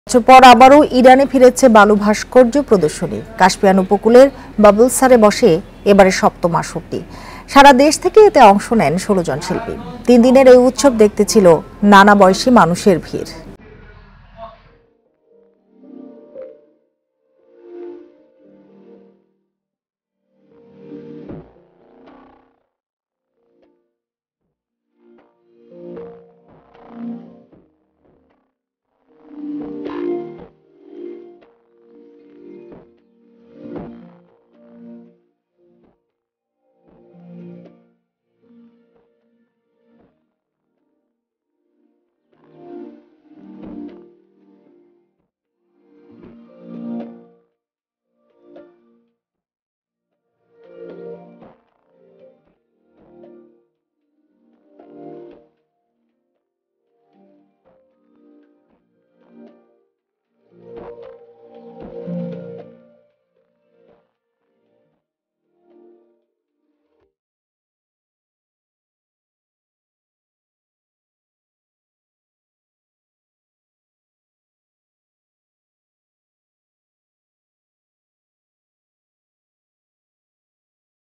ইরানে ফিরেছে বালু ভাস্কর্য প্রদর্শনী || Iran Sand Sculptures end show
১২ বছর পর আবারো ইরানে ফিরেছে বালু ভাস্কর্য প্রদর্শনী। কাস্পিয়ান উপকূলের বাবোলসারে বসে এবারের ৭ম আসর। সারা দেশ থেকে এতে অংশ নেন ১৬জন শিল্পী। তিন দিনের এই উৎসব দেখতে ছিল নানা বয়েসি মানুষের ভিড়।